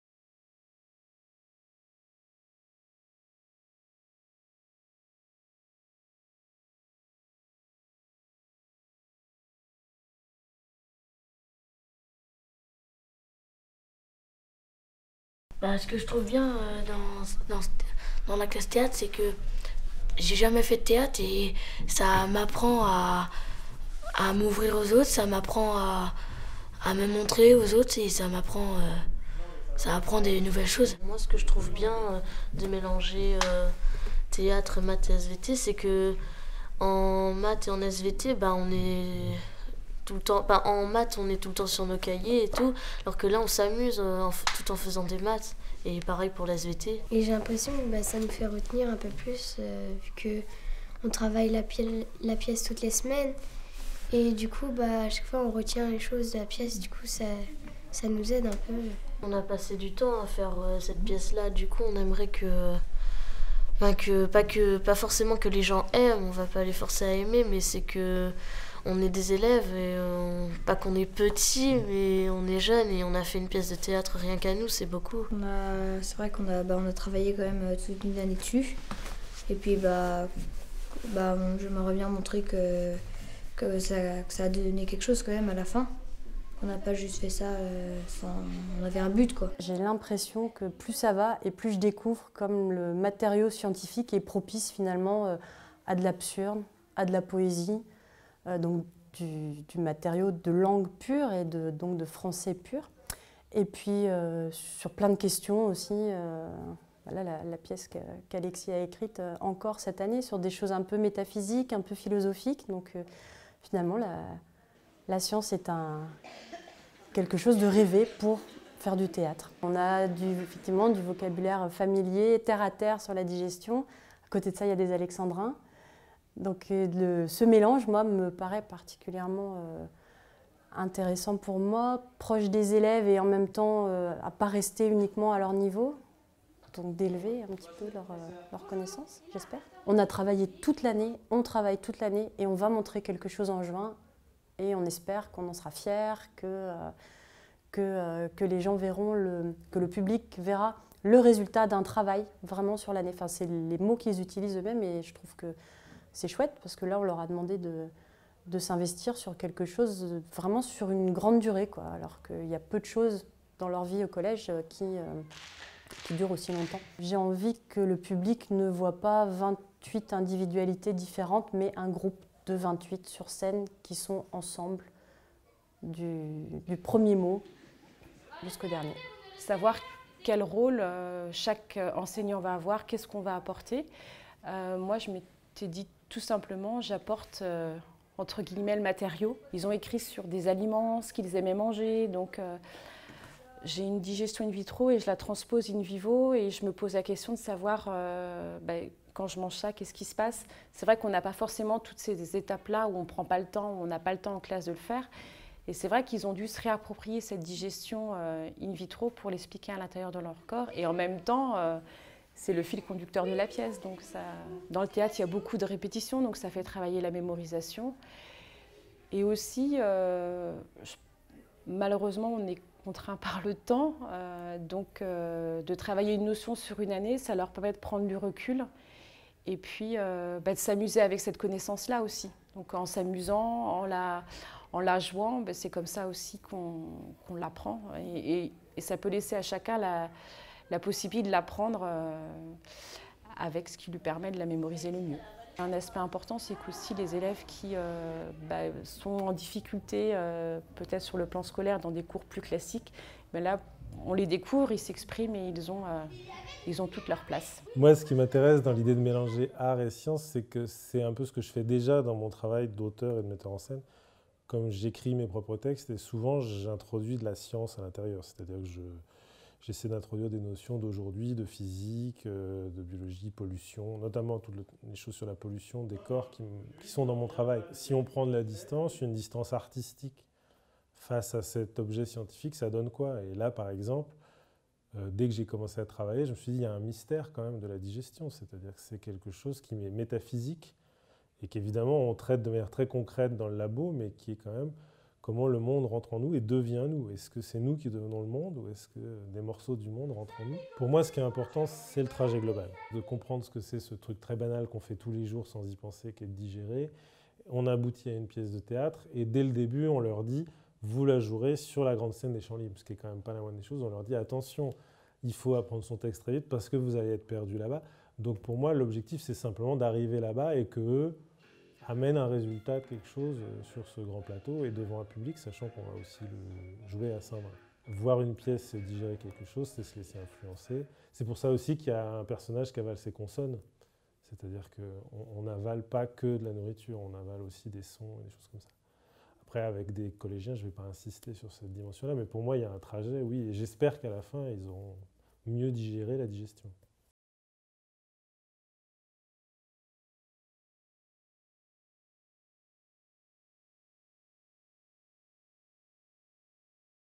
La science sur les planches 2015 - Interview Saint-Méen-le-Grand
Dans le cadre du projet « la science sur les planches », l'Espace des sciences a interrogé tous les participants (élèves, enseignants, intervenants artistiques) du collège Camille Guérin de Saint-Méen-le-Grand pour le projet intitulé « Ça commence par la faim », une création originale pour faire découvrir les sciences autrement !